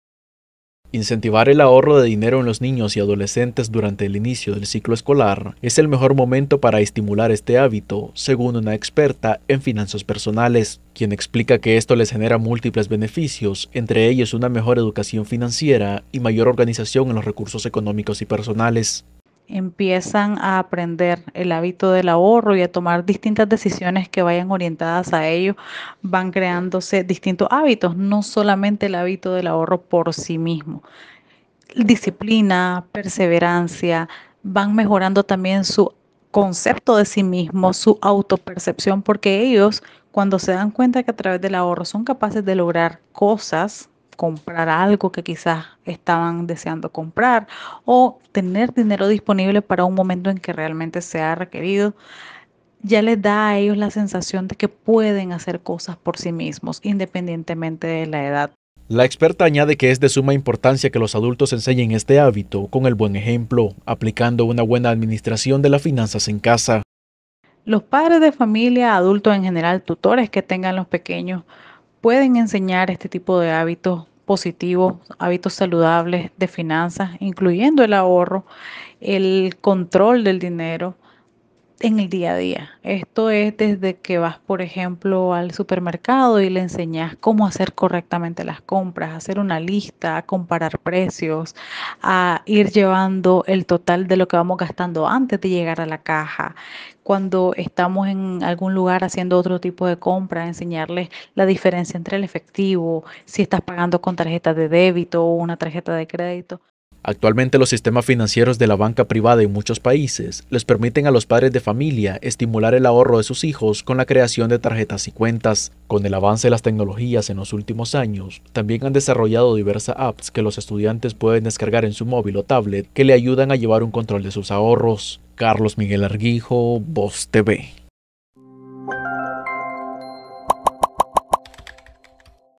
La importancia de incentivar el ahorro en los niños
Incentivar el ahorro de dinero en los niños y adolescentes durante el inicio del ciclo escolar, es el mejor momento para inculcar este hábito, según explica Gissela Canales, experta en finanzas personales.